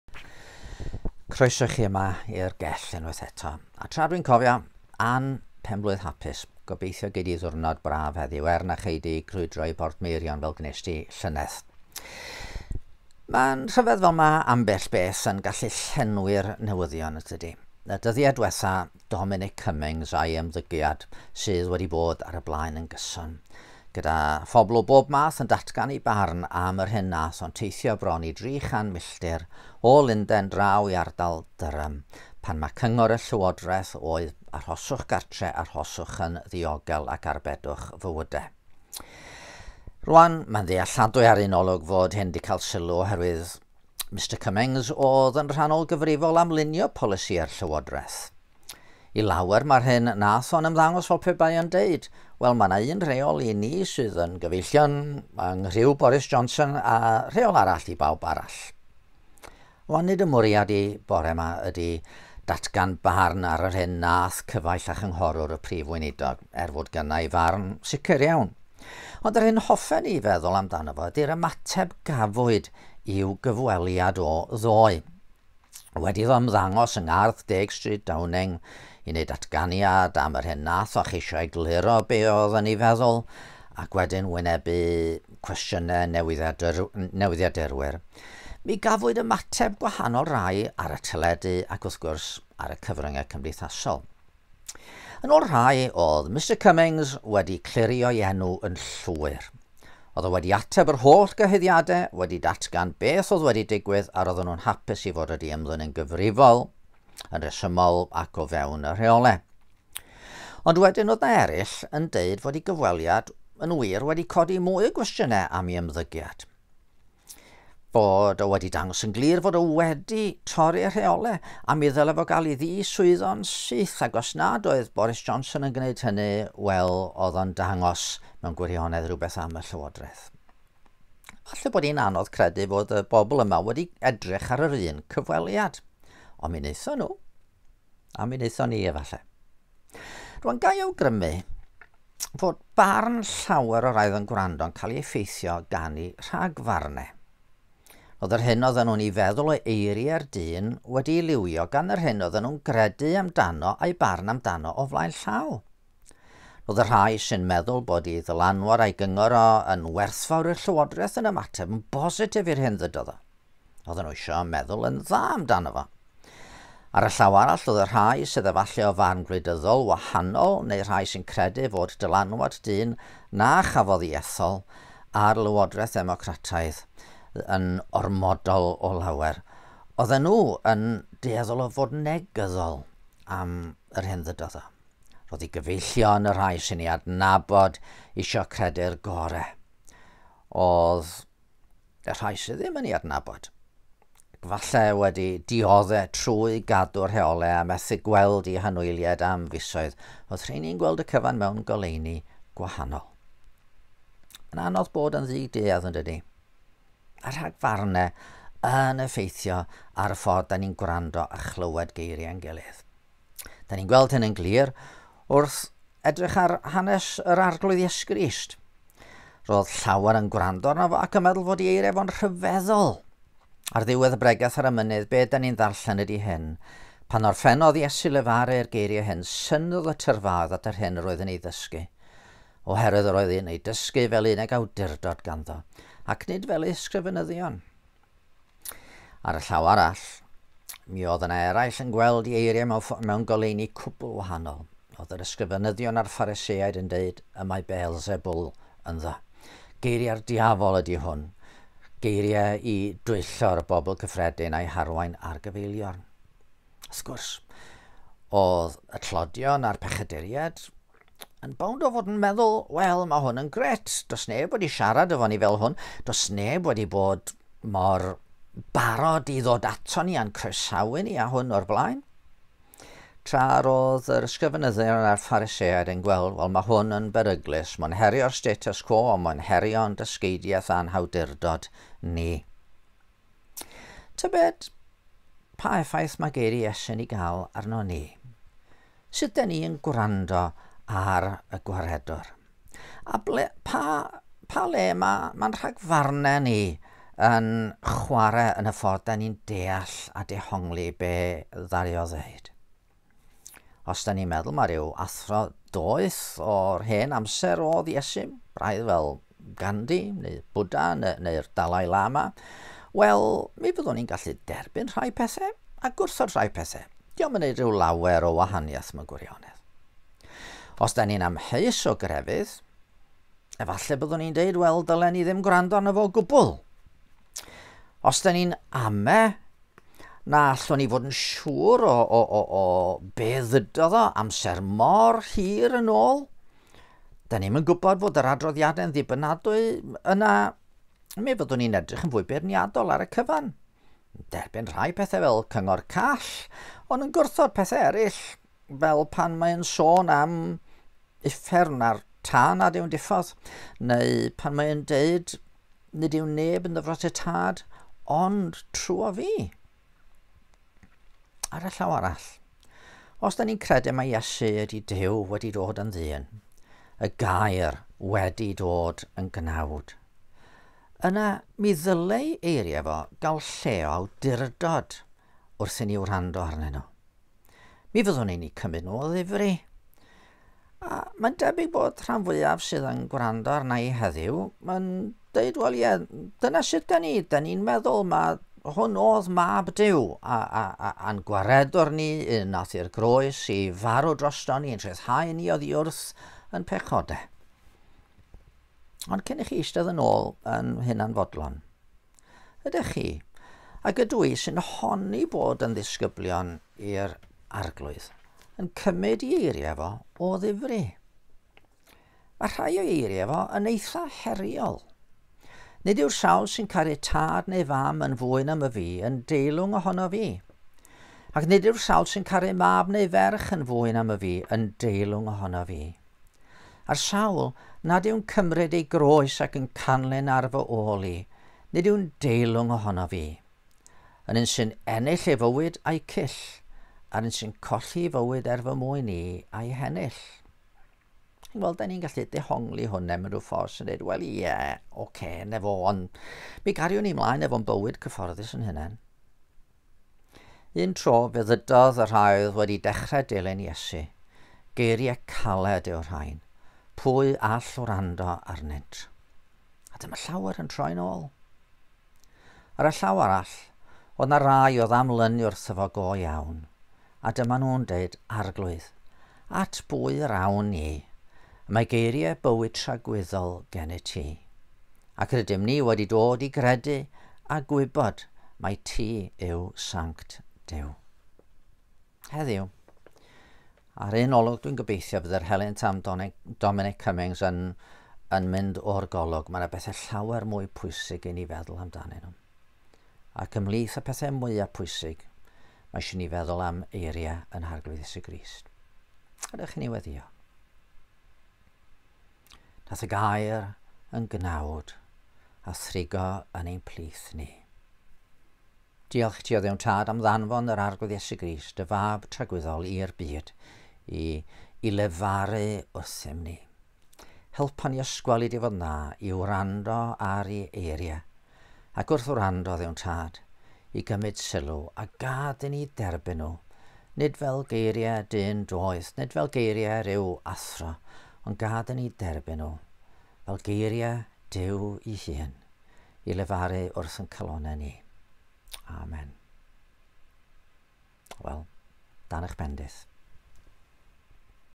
Gair o'r Gell 46
Yn dilyn ymateb pobl i gyfweliad Dominic Cummings ddoe, sut mae'n rhagfarnau yn effeithio ar y ffordd fyddwn ni'n gwrando? Myfyrdod Dafydd Job